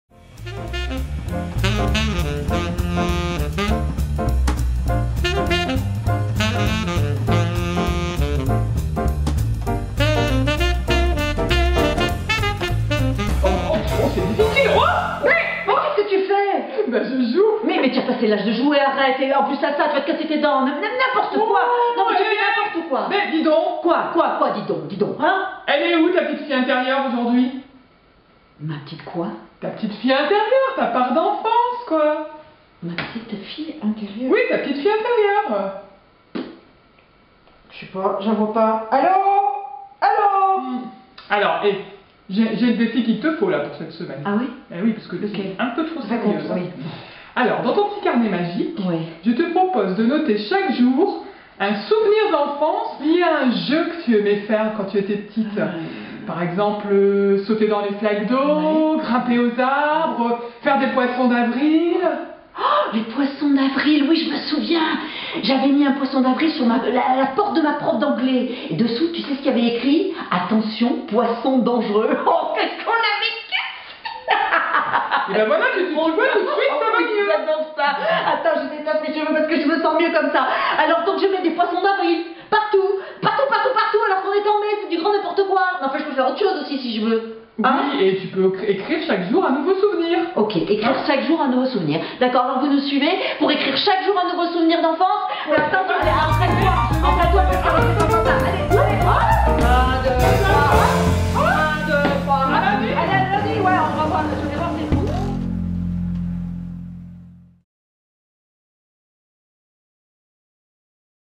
le petit défi du lundi n°7: retrouver sa part d'enfance
Isabelle et Laurence vous proposent aujourd'hui d'amener de la fantaisie dans votre vie en renouant avec votre enfant intérieur...